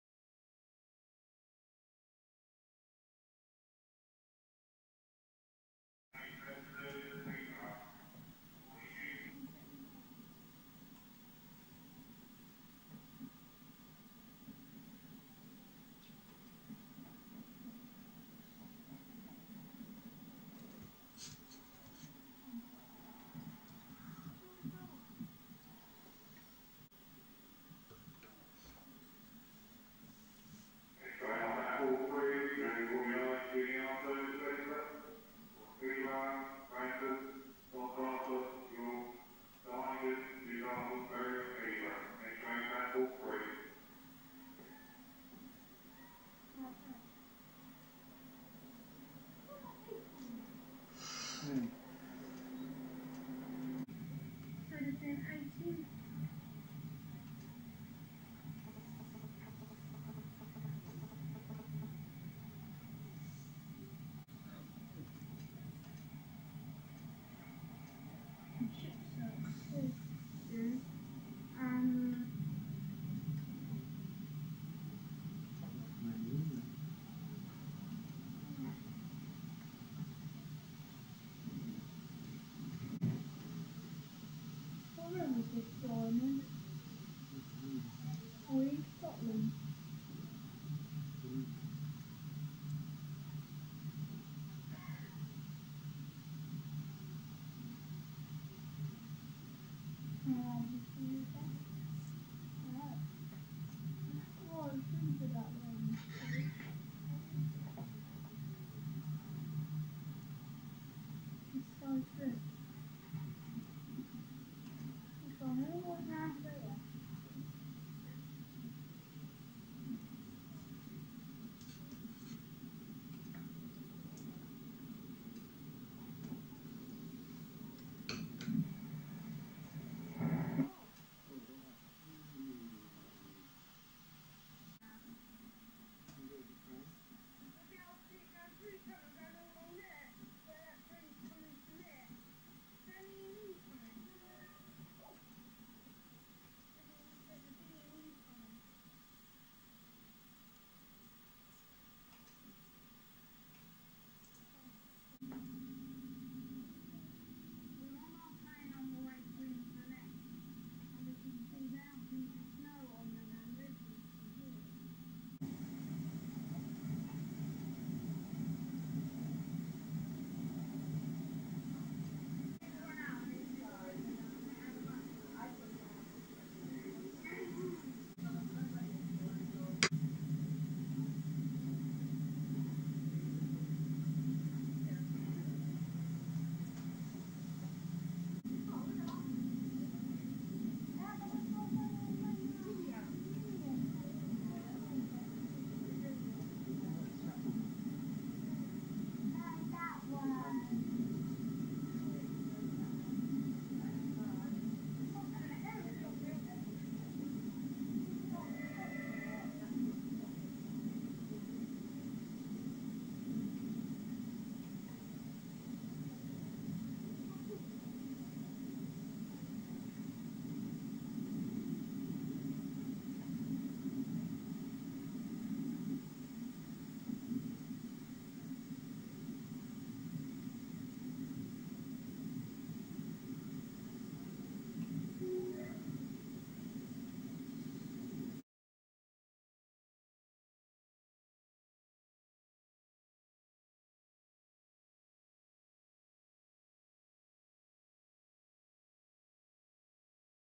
A trip to Scotland in April 2000
Henry's Adventures Archives - Episode 26.

Starting at Stevenage Station we see a class 365 and class 317 both still in Network South East livery. A GNER class 91 arrives, we enjoy videos from the Royal Boarder Bridge at Berkwick-upon-Tweed before arriving at Edinburgh where we see some trains from the top of Scot Monument with Edinburgh Castle in the background.

We then see views from the train on the Highland Main Line, the Kyle Line and the Forth Bridge.